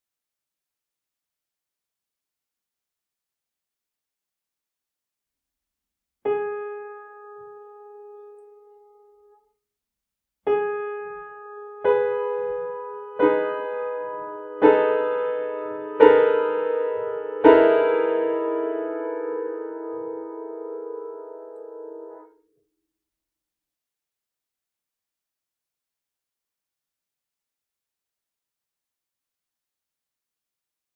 Cluster, lesson 136_Cluster, Aufgabe 136 | Ear training | Gehörbildung
Cluster

In this lesson you have to figure out a cluster, which is building up note by note – with every attack one note is added. You only have to write the notes, which are added and not the whole chords. Good luck!

Cluster

In diesem Aufgabentyp hört ihr ein Cluster, das Ton für Ton aufgebaut wird – mit jedem Anschlag wird ein Ton hinzugefügt. Ihr müsst nicht jeweils den ganzen Akkord notieren, den ihr hört, sondern nur den Ton, der bei jedem Anschlag dazukommt. Viel Erfolg!